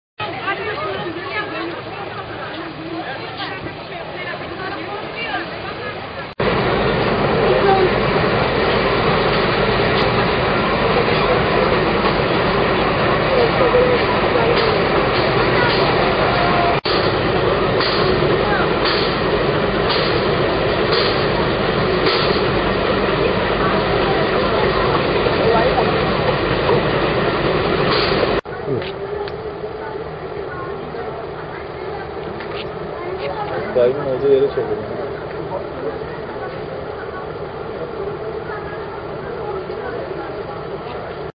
tarsus'ta 2 katli evde meydana gelen yangin
Mersinin Tarsus ilçesinde meydana gelen yangın mahalle sakinlerine korku dolu anlar yaşattı..